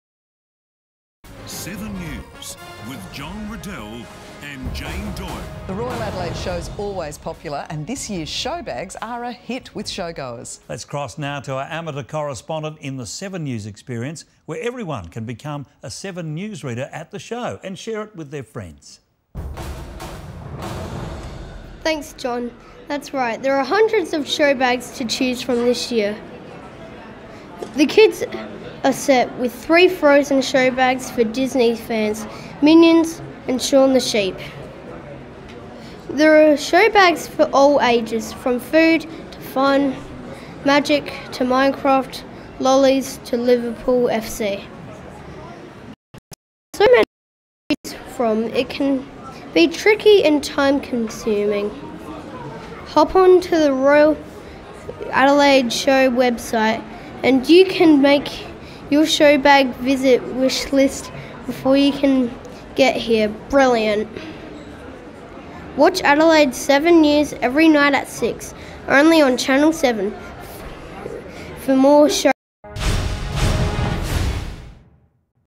Brendon M - 7 News Experience
Exclusive report from the Royal Adelaide Show by: Brendon M